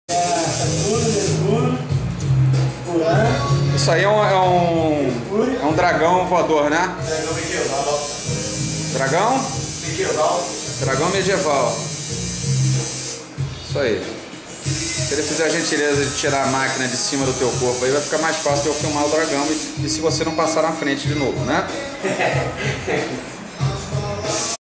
Renan Silva... Dragão Medieval... Parte II...
Parte II - Renan Silva e seu irado "Dragão Medieval", sendo brilhantemente tatuado por Marquinho Tattoo, do AQUÁRIUS STUDIO ( , em Resende, RJ...